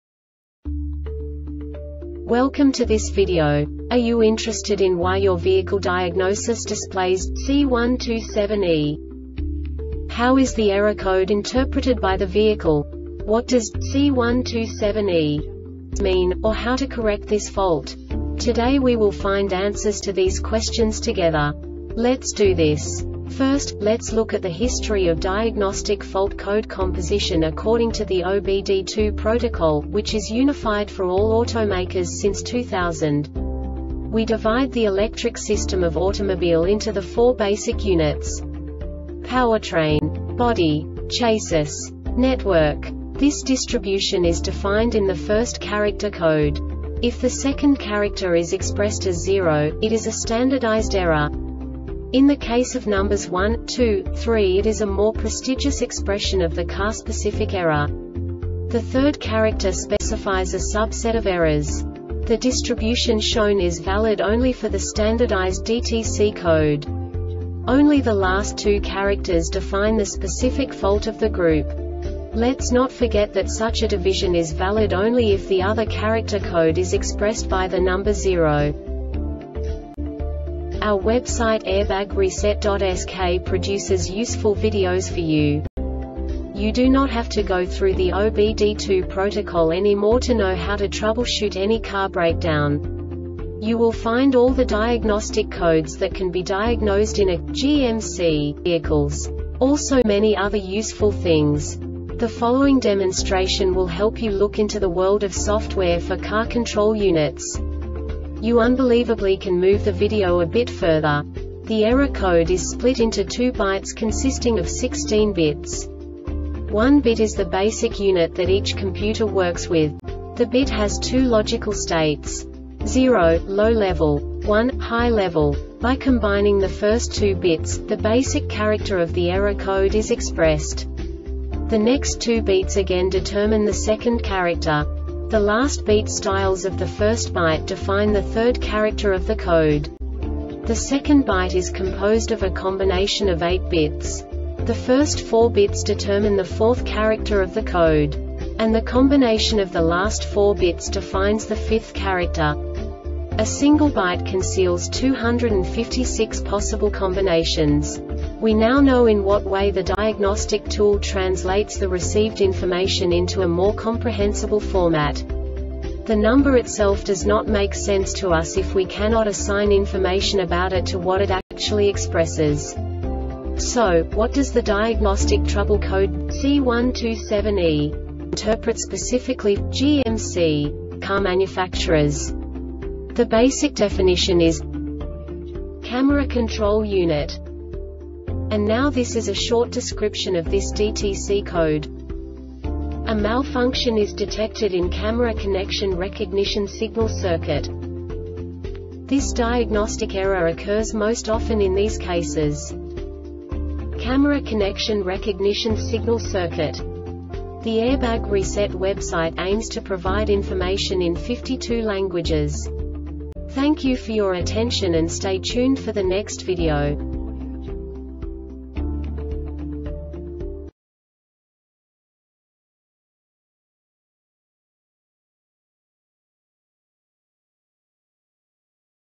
DTC GMC C127E Short Explanation
Contents:
0:21 Basic DTC analysis according to OBD2 protocol standard.
1:48 Insight into programming
2:58 A diagnostically understandable description of the Diagnostic Trouble Code
3:05 Basic error definition

Make:
GMC
Code:
C127E
Definition:
Camera Control Unit
Description:
A malfunction is detected in camera-connection recognition signal circuit.
Cause:
Camera-connection recognition signal circuit.